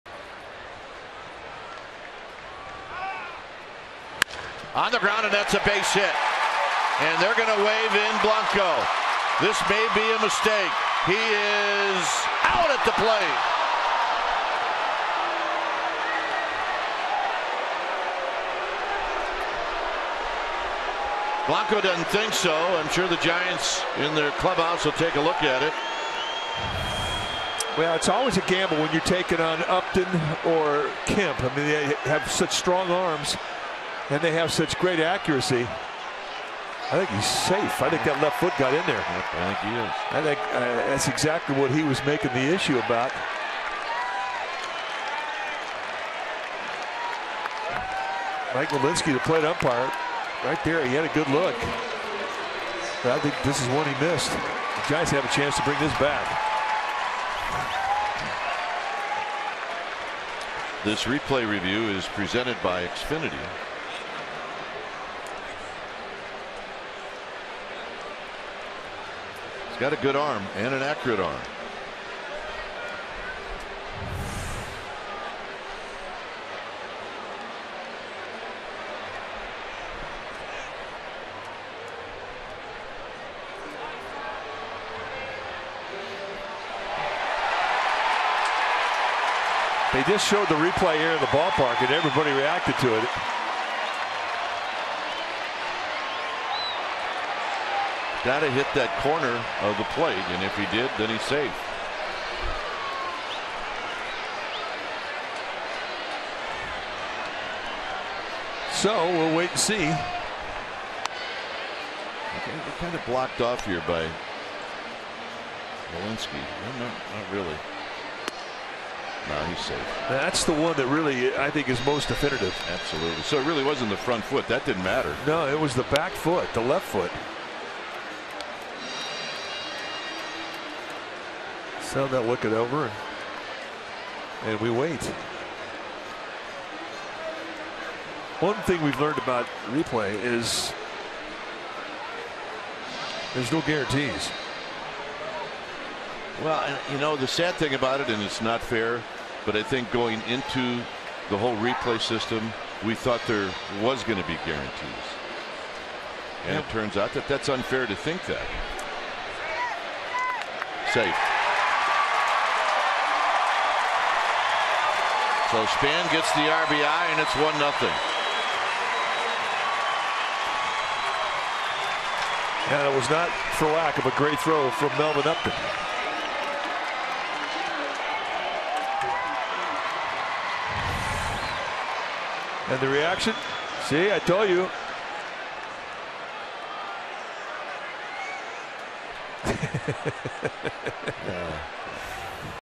SD@SF: Span plates Blanco after call overturned
Gregor Blanco is called out at home on Denard Span's single in the 3rd, but after the Giants challenge, the call is overturned